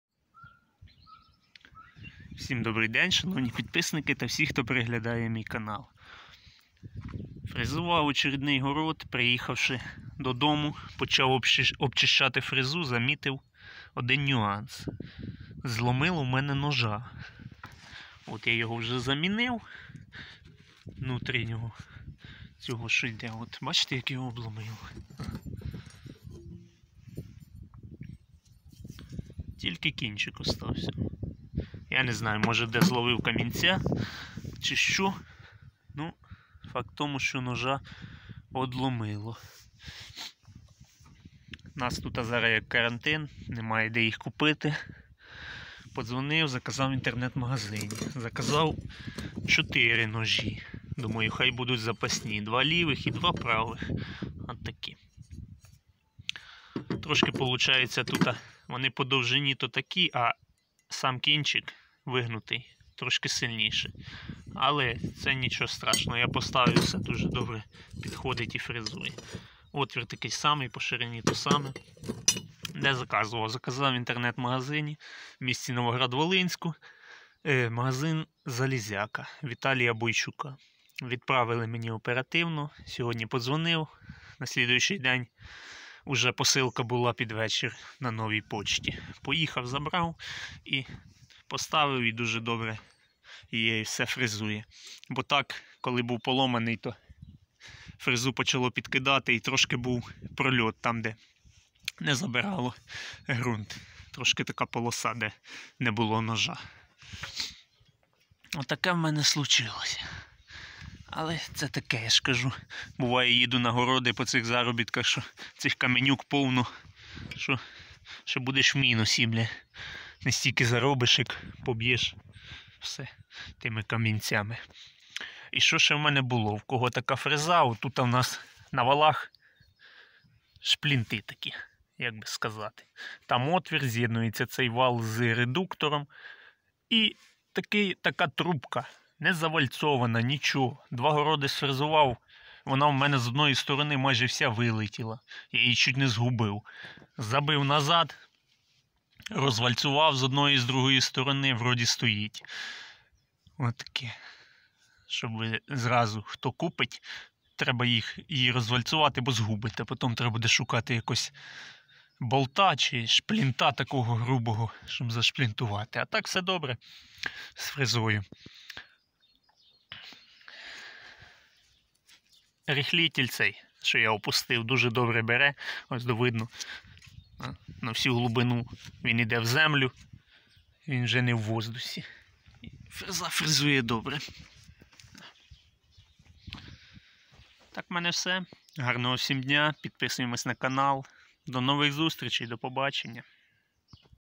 Зломило ножа грунтофрези 1.4м, Шифенг 350l
Купував ножі запасні у Віталія Бойчука, м. Новоград-Волинськ.